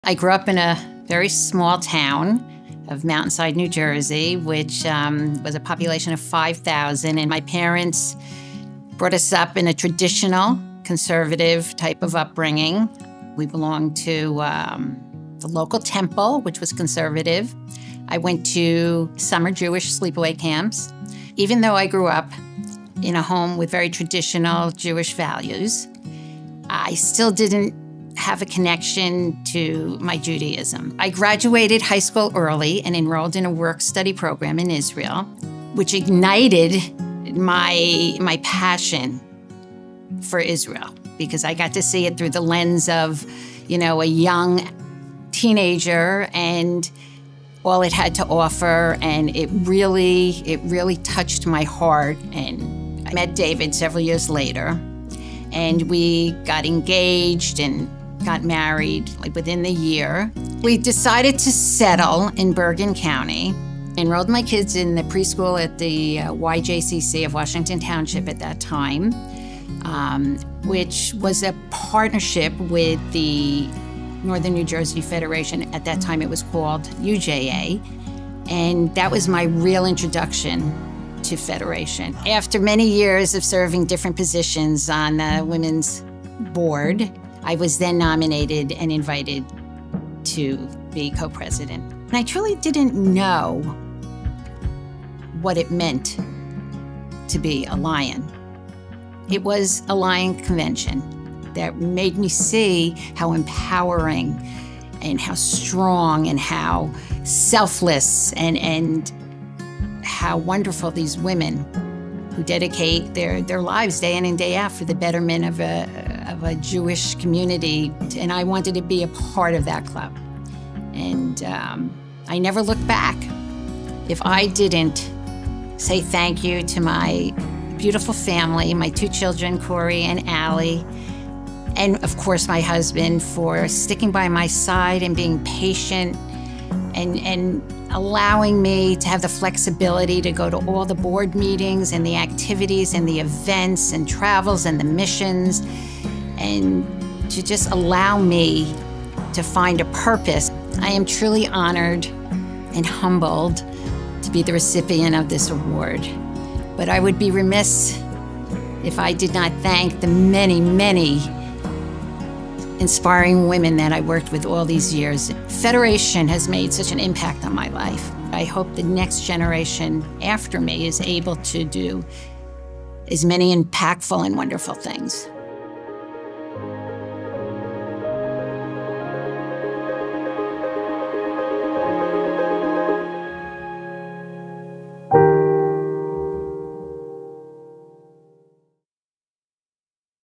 Jewish Federation of Northern New Jersey Spring Luncheon 2024 - Geri Cantor
Geri Cantor - Women’s Philanthropy Past President - Women's Philanthropy Spring Luncheon 2024